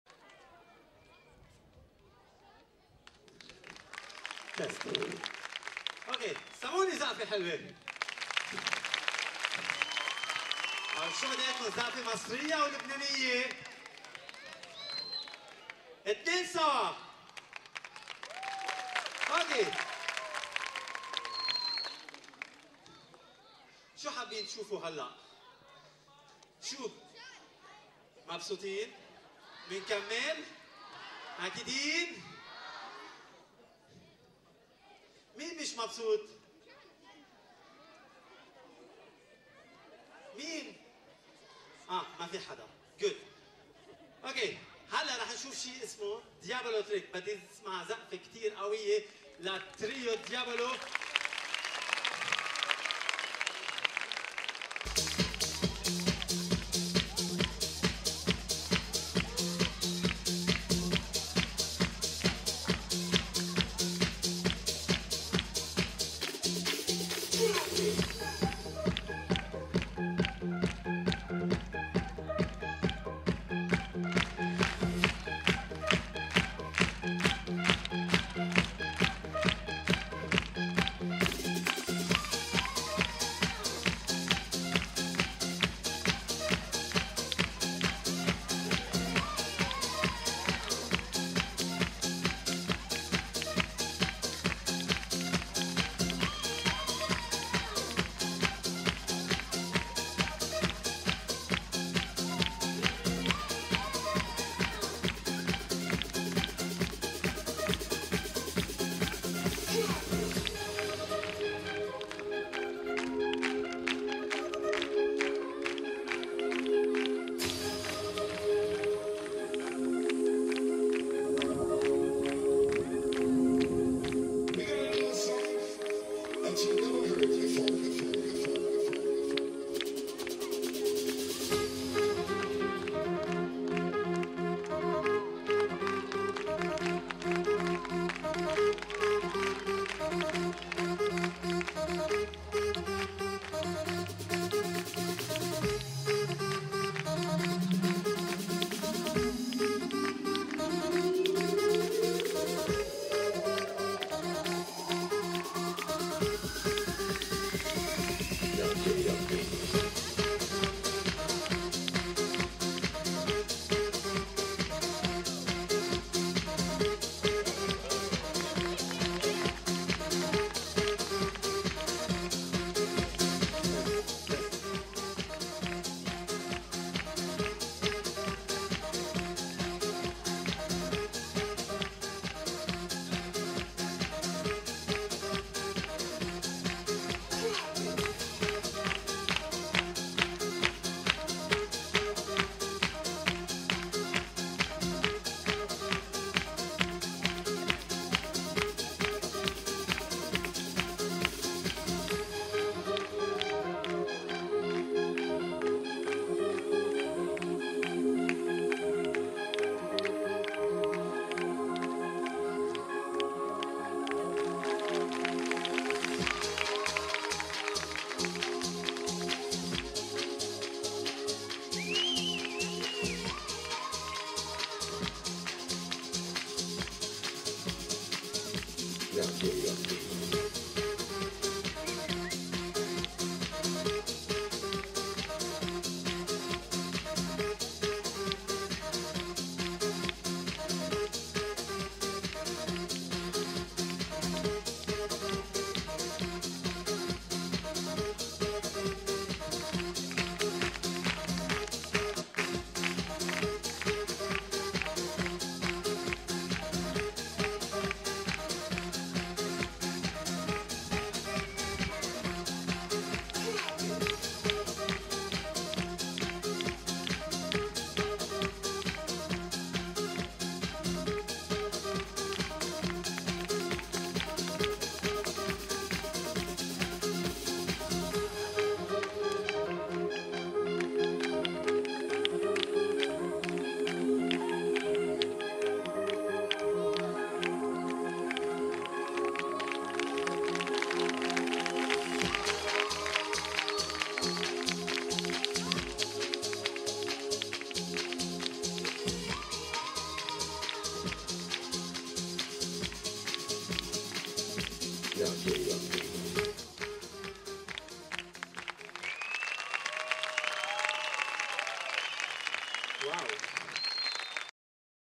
Circairo 2010 - Eirque de liban lebanon | مهرجان القاهرة الدولي للسيرك
Eirque de liban lebanon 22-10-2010

تنظم مؤسسة المورد الثقافي بدعم من مفوضية الاتحاد الأوروبي في مصر وقطاع العلاقات الثقافية الخارجية- وزارة الثقافة وهيئة تنشيط السياحة أول مهرجان دولي لفنون السيرك في مصر